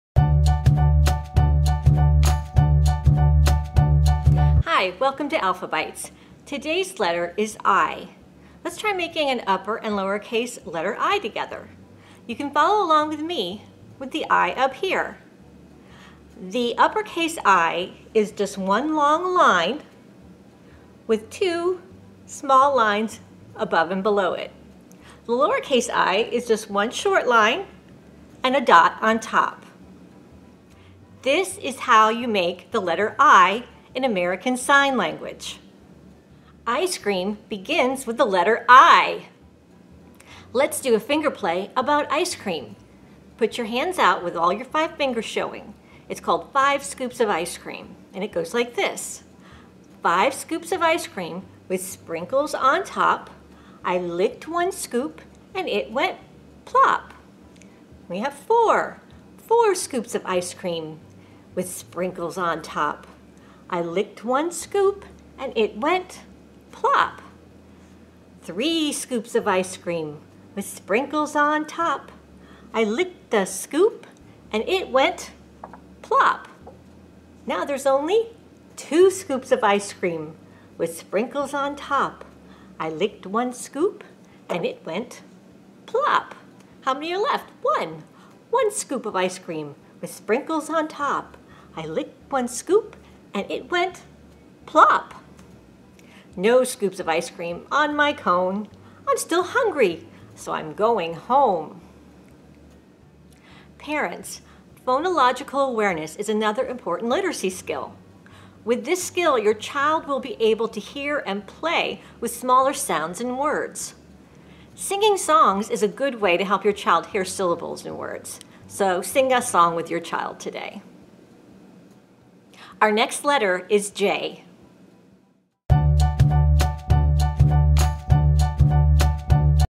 Alphabites I
Welcome to Alphabites!  A series designed to bring bite sized letter fun and literacy to you and your child. Each video showcases a letter you'll learn how to make in American Sign Language and trace in upper and lower case form.  Then, stick around for songs, fingerplays and literacy tips!

Filmed at the Main Library, 100 N Osceola Ave, Clearwater, FL 33755.